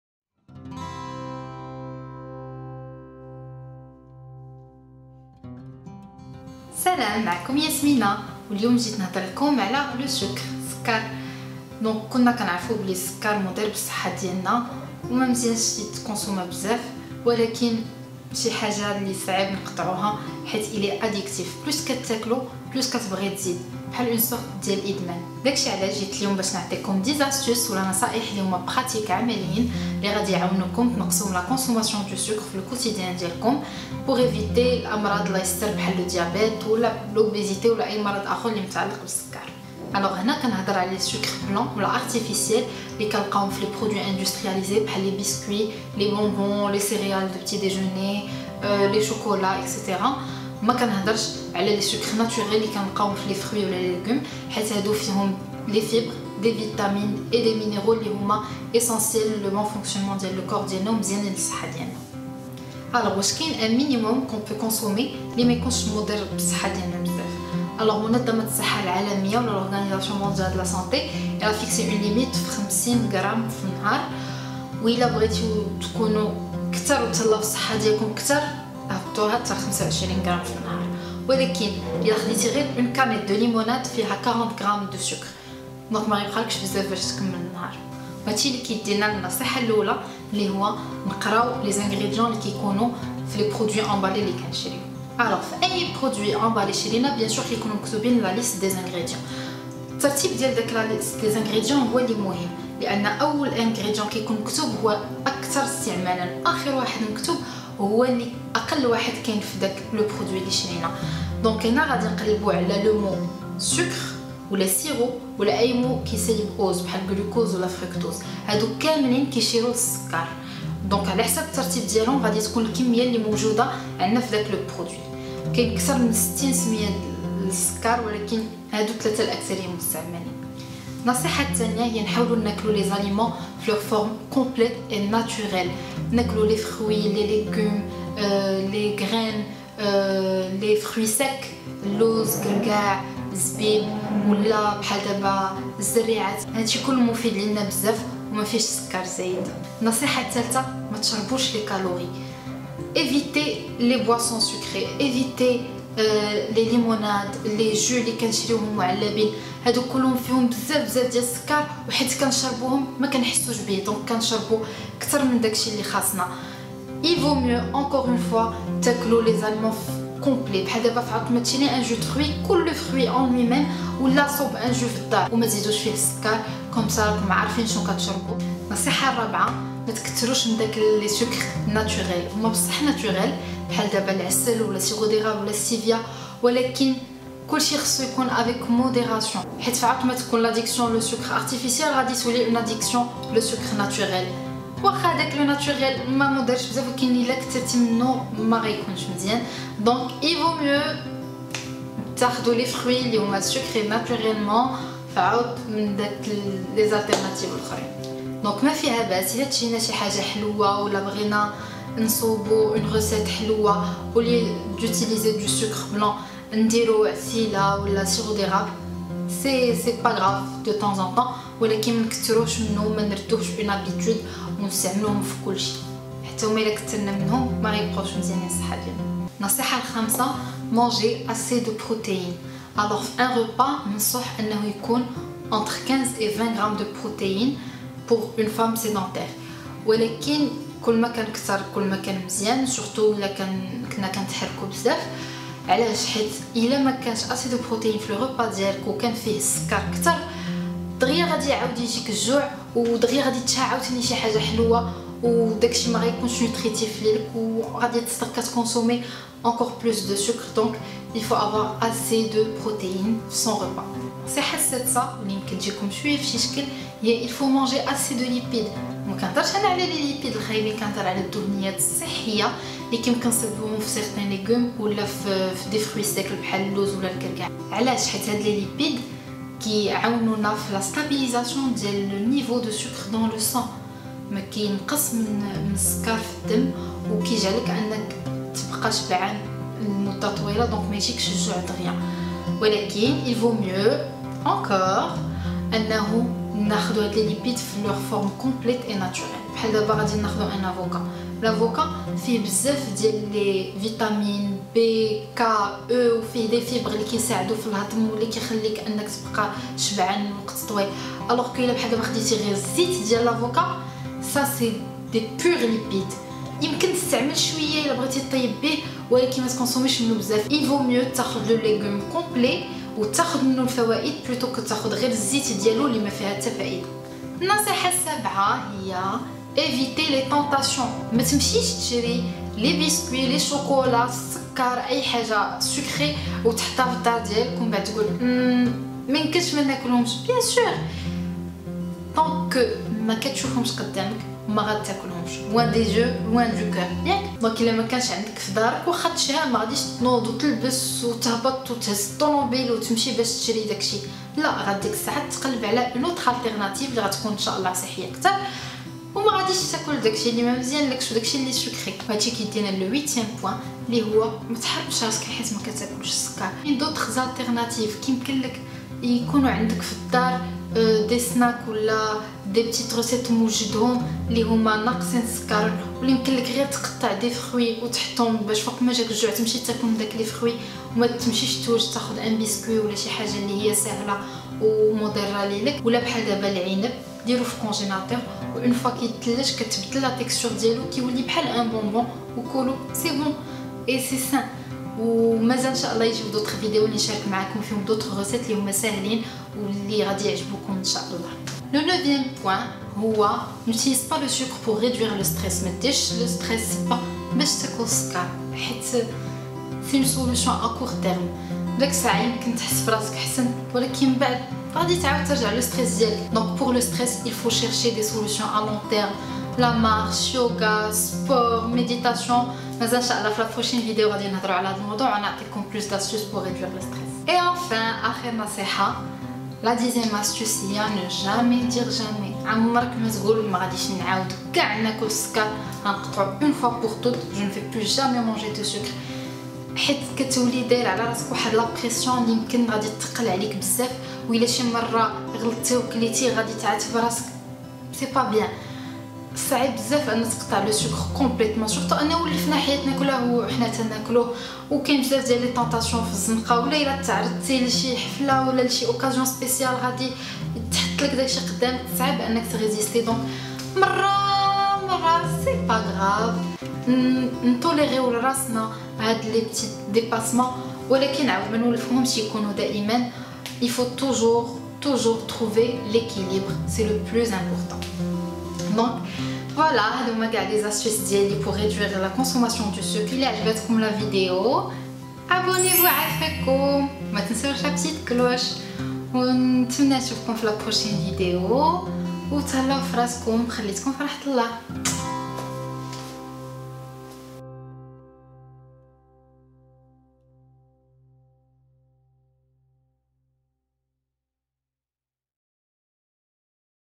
10 tips to reduce sugar intake -نصائح للنقص من استهلاك السكر
Salaaam ! C'est Yasmina

Bienvenue sur ma chaîne :) aujourd'hui je reviens avec une nouvelle vidéo et un sujet que j'estime très important.

J'espère que ces conseils vous aideront à mieux prendre soin de votre santé. Si vous avez d'autres astuces, n'hésitez pas à les partager avec nous en commentaire pour que tout le monde puisse en profiter !

(je m'excuse du bruit entre la minute 2 et 3, les voisins ...)

Je vous remercie pour votre soutien et j'espère vous voir lors de ma prochaine vidéo :)